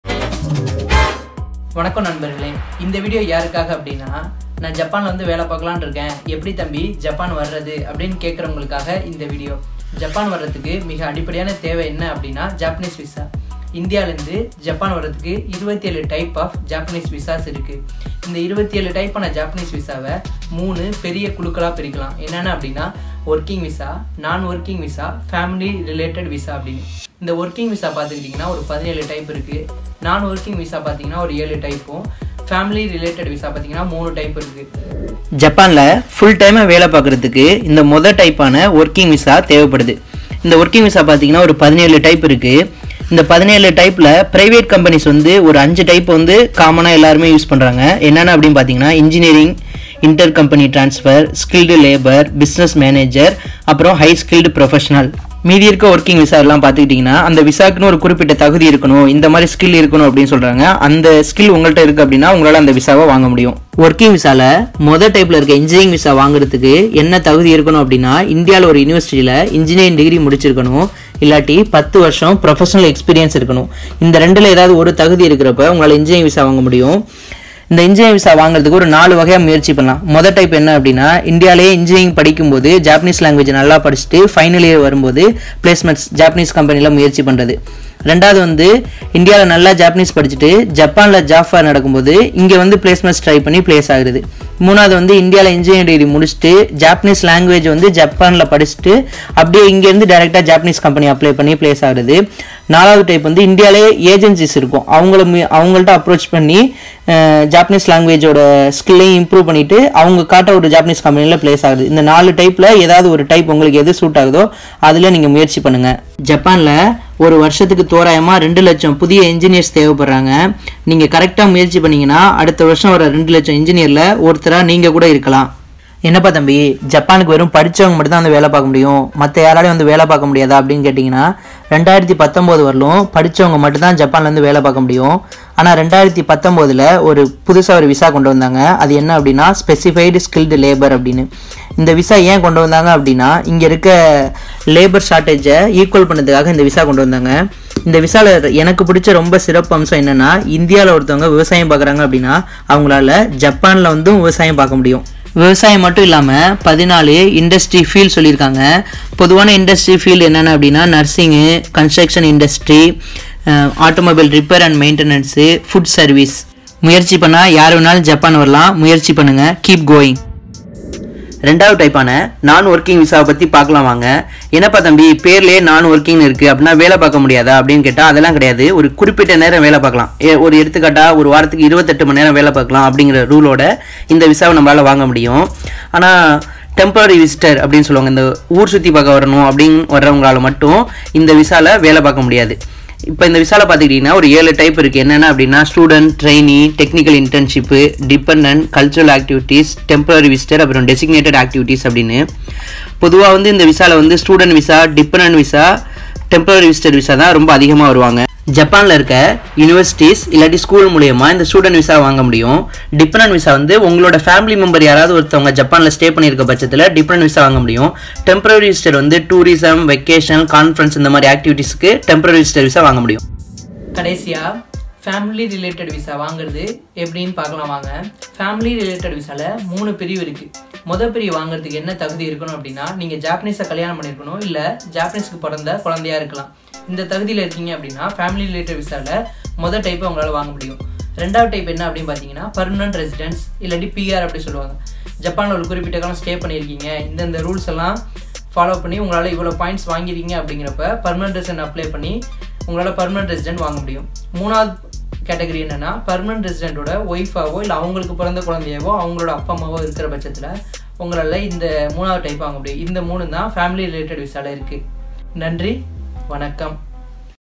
ஜப்பான் விசா|How to get a Japanese visa In Tamil
Detail explanation of Japanese visa in Tamil.